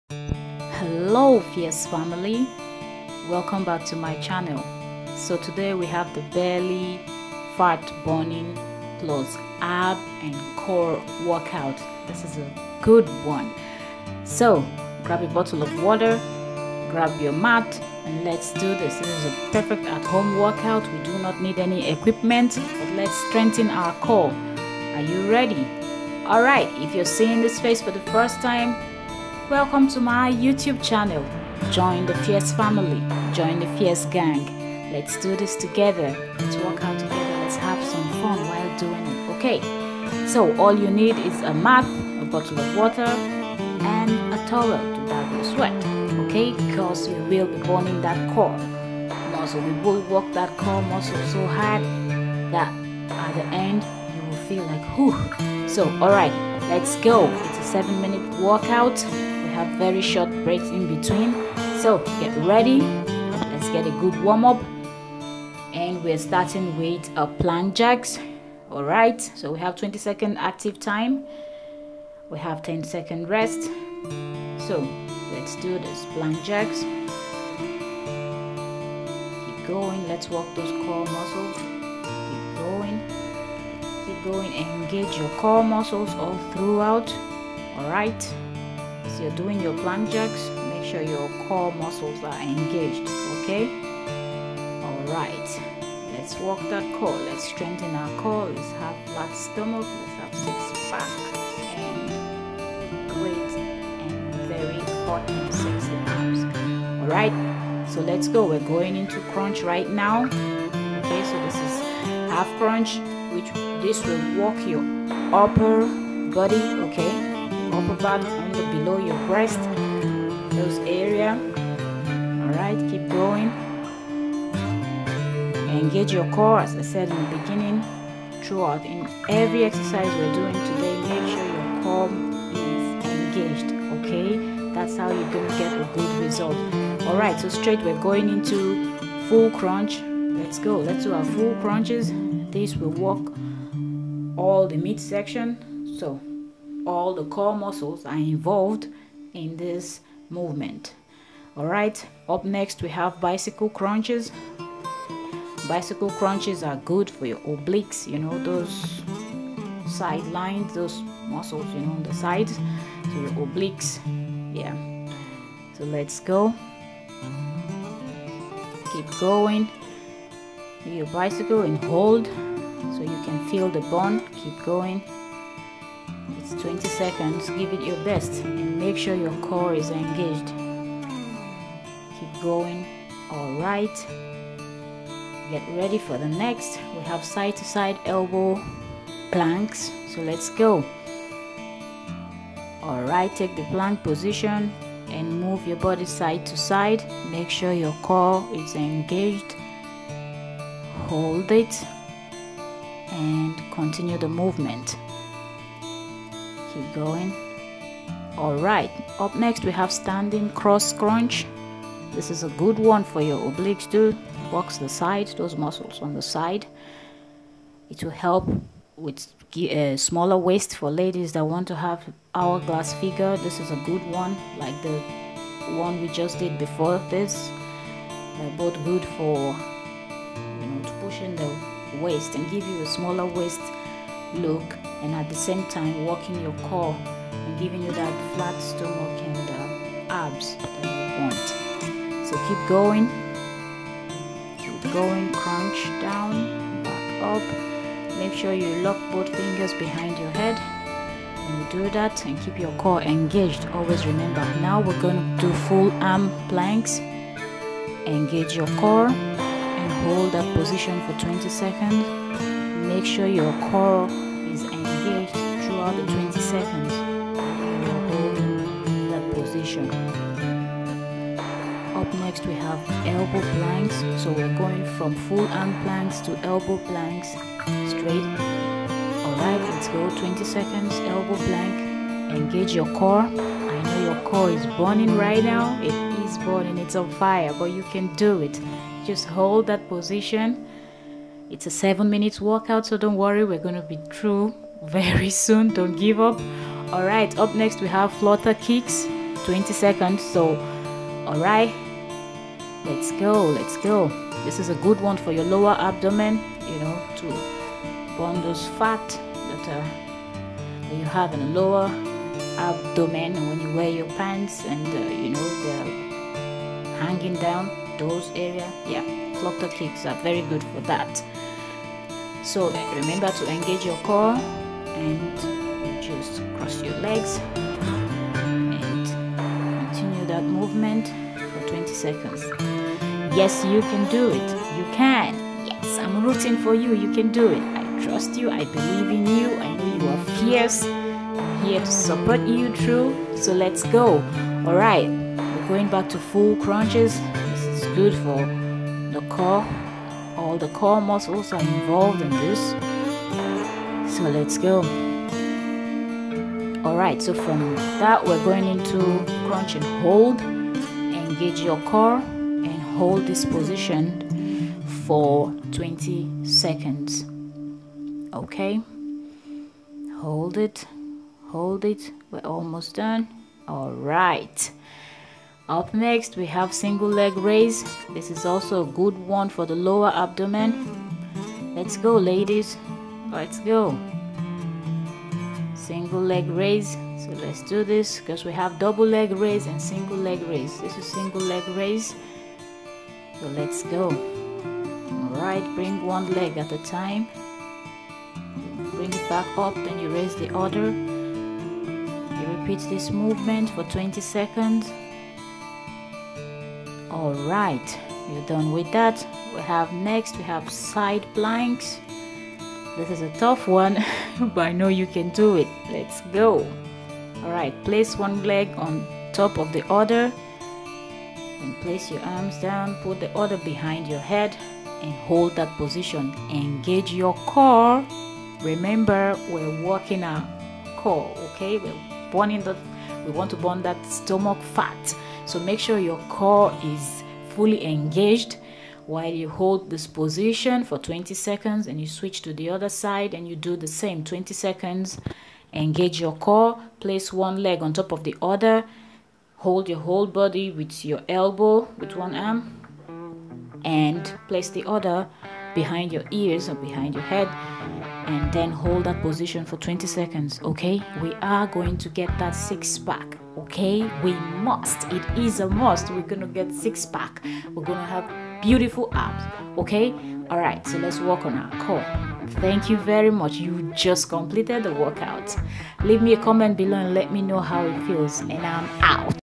BELLY FAT BURNING +ABS WORKOUT AT HOME/STRENGTHEN YOUR CORE IN 7 MINUTES
This belly fat burning and abs workout at home will not only help you burn belly fat but will also strengthen your core and leave you looking hot.in order to start seeing difference in your belly area do this workout two to three times in a week but remember to allow yourself to rest too.these exercises are highly effective and it is suitable for everyone as it is beginner friendly